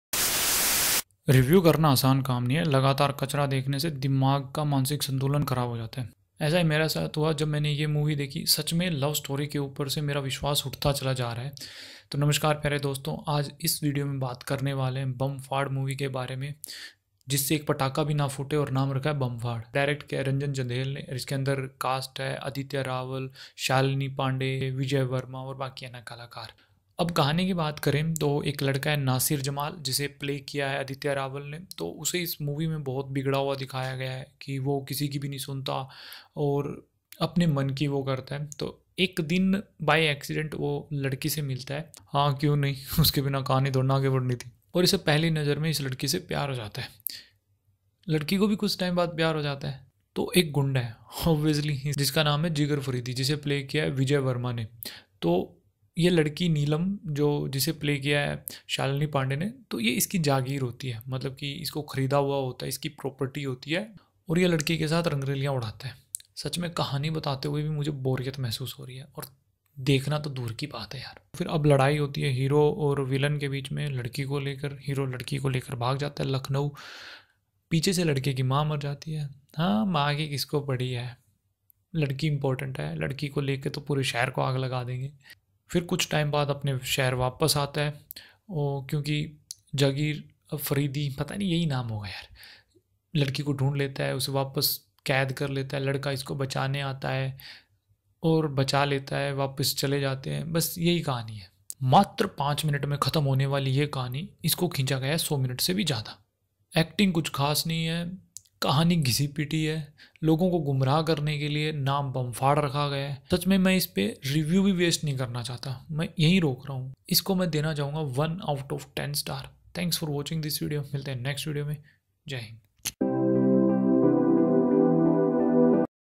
Bamfaad Movie Review | Aditya Rawal, Shalini Pandey, Vijay Varma
A passionate love story set in Allahabad between Naate and Neelam who meet accidentally and fall in love. They make brave choices and go through a path of love and loss to be with each other.